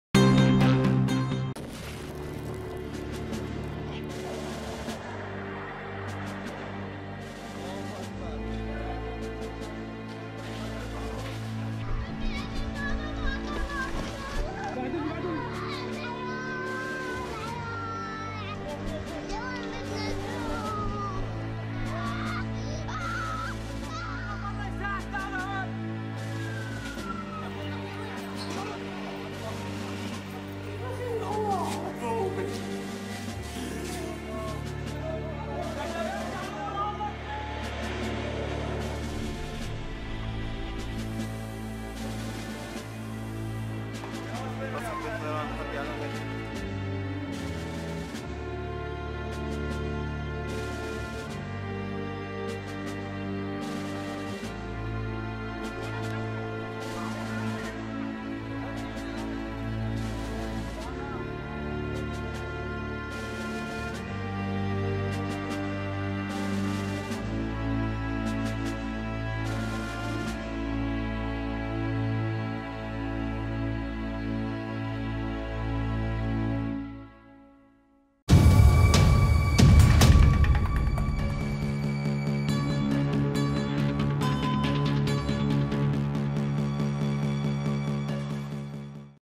Picture This | Syria’s Decade of Devastation
Monday marks 10 years since an uprising began in Syria. Here are some of the most striking images from the decade-long war.

Video produced by Irem Koca.